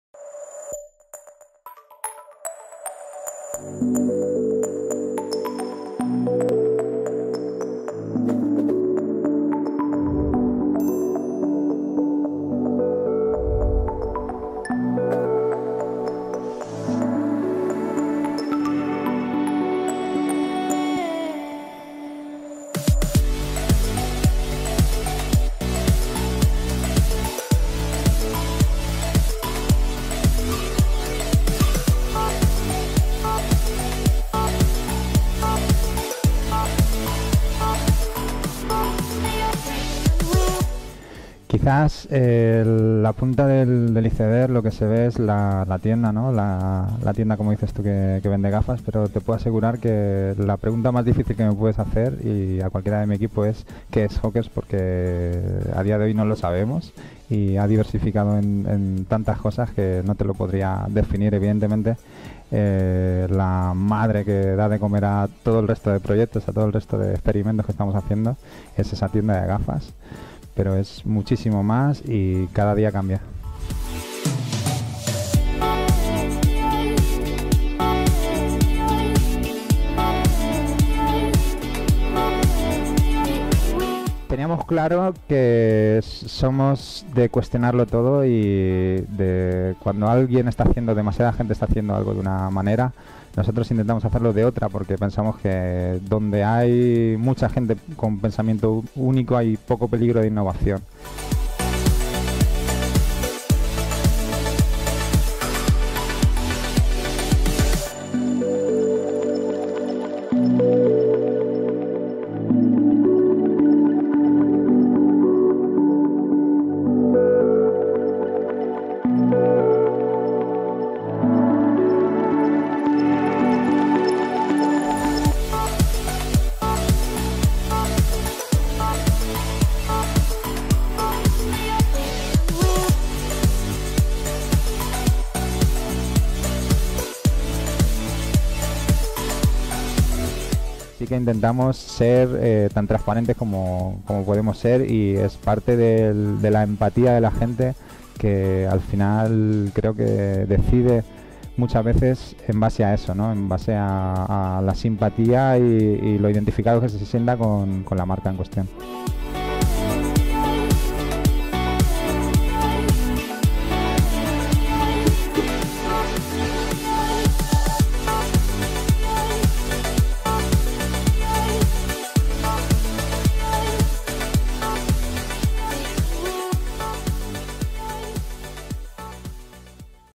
David Moreno: "Hawkers is much more than just an eyewear store"
In December 2013, four friends from Elche revolutionised the sunglasses industry by setting up a new company. How?

Selling quality eyewear at a good price by making themselves known through social networks.

On October 25th we discussed with one of the Hawkers founders their success story and we discovered how to turn clicks into purchases.